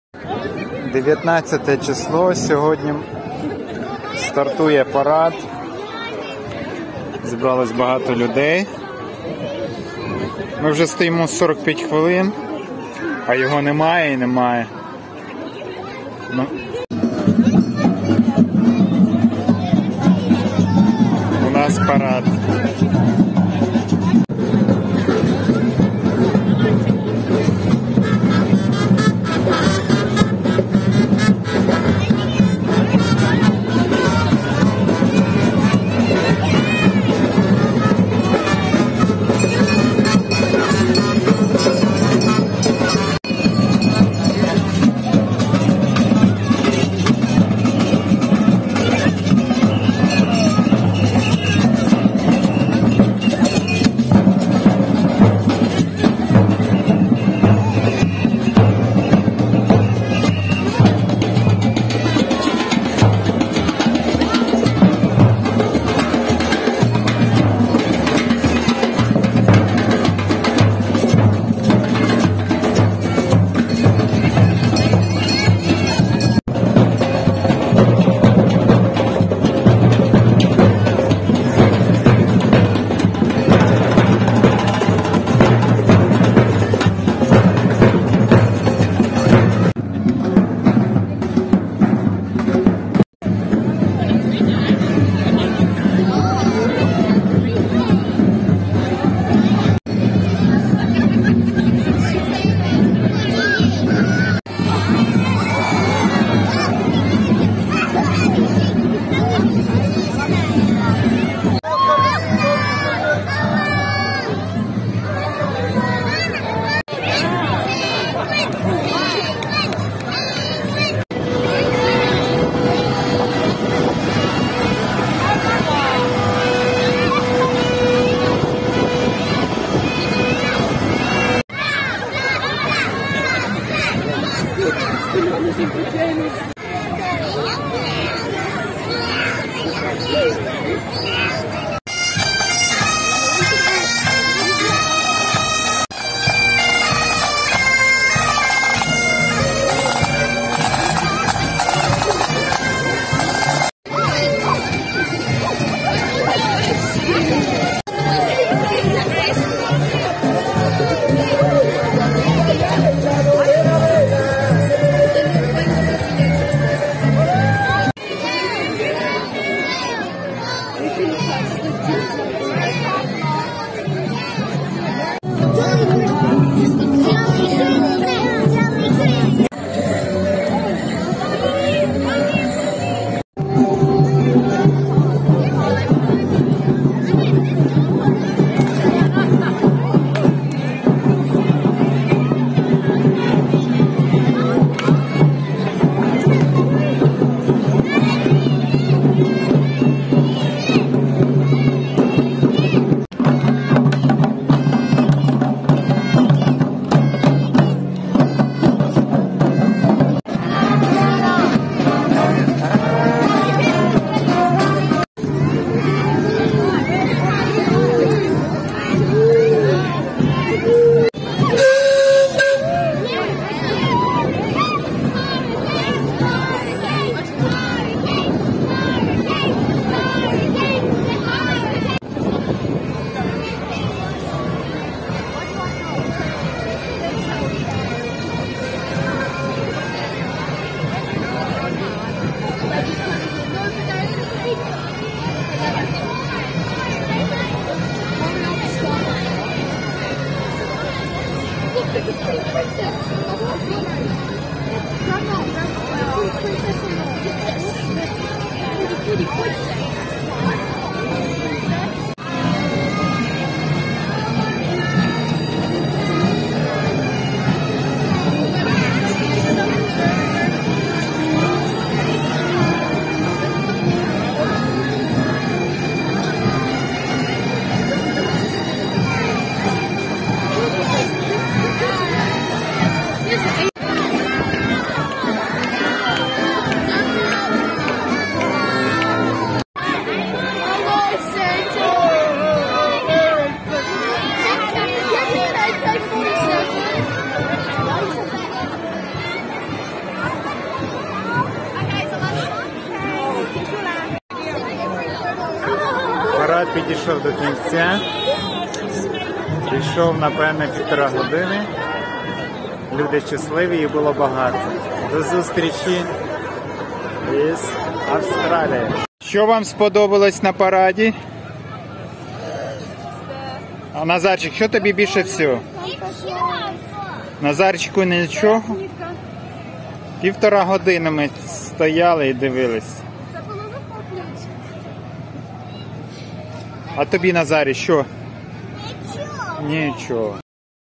Австралія. Новорічний парад в Тасмании. 19.11
Tasmanii. Дуже цікаве шоу. Австралія цікава країна.